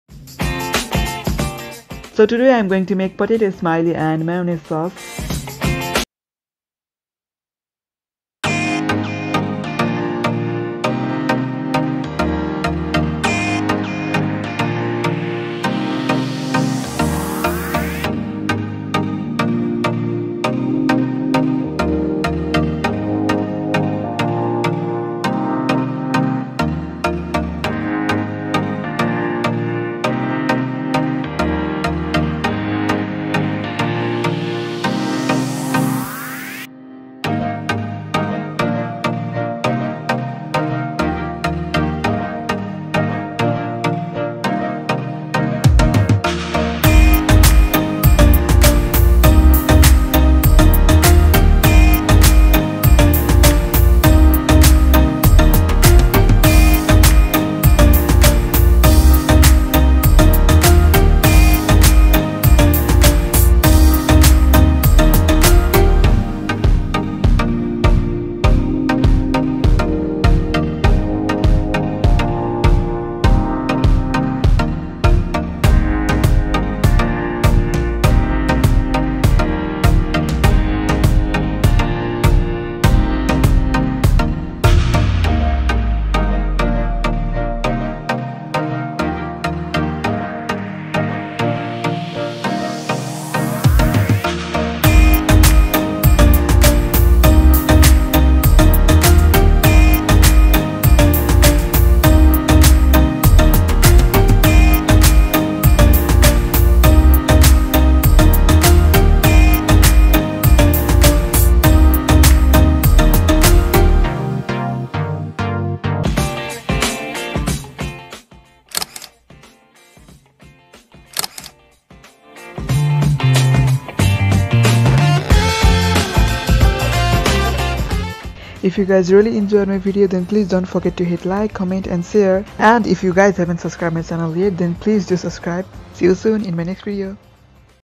My 1st cooking vlog 😅 I can cook too 🤤
Ritik_rai latest Ep2 vlog recipe art&soul ritikrai latest Cook some Homemade Mayonnaise sauce & tasty potato smiley - & it's very quick & easy .So what you are waiting for - go to your kitchen - Make it - taste it & enjoy it.

 Also follow Thanka  Yatra page on FB